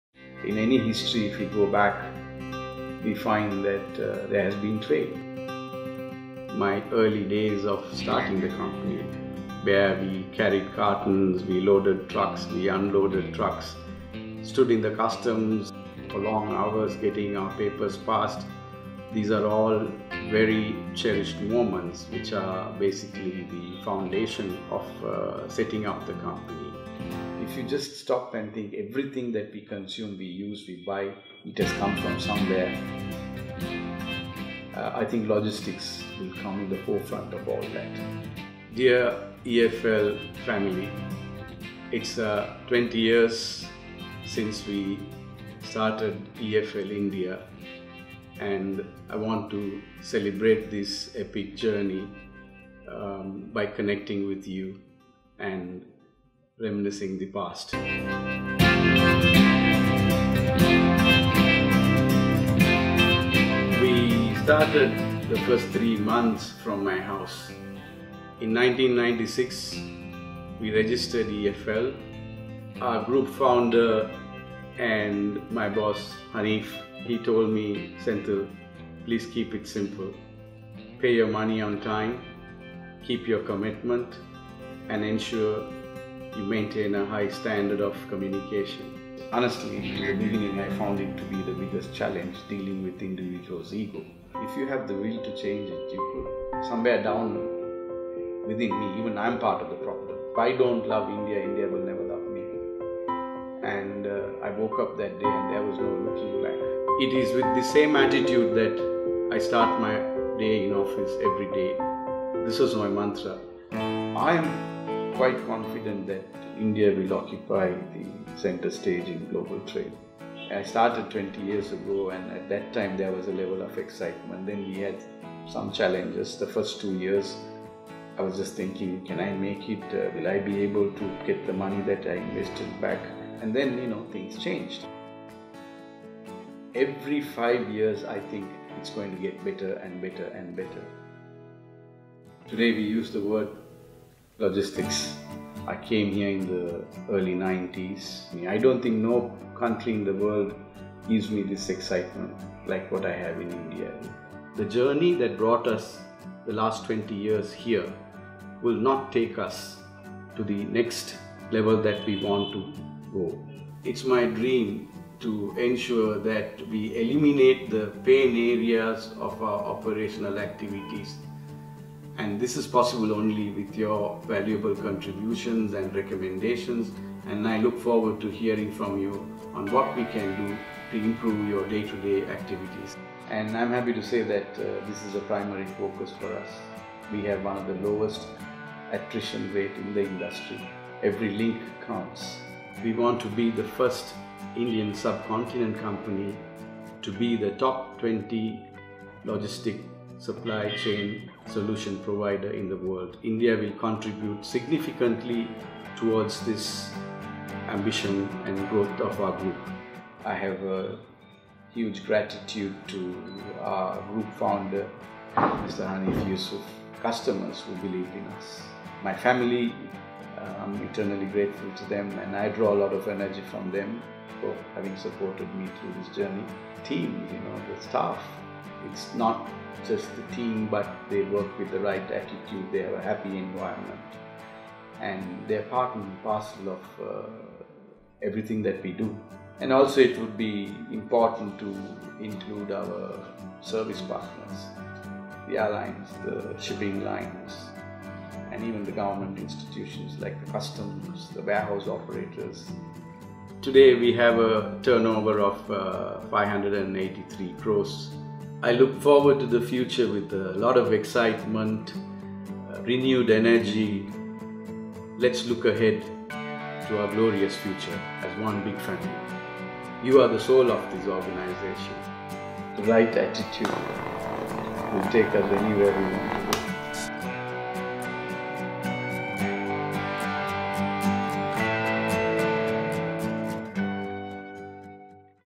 EFL | Logistic company | 20th Anniversary | Look Back Documentary | 2016
EFL, a dynamic Chennai Based Logistic firm turning 20. The founder Mr.Senthil talks about his and the company's journey to become one of the fastest-growing Logistics Provider in India.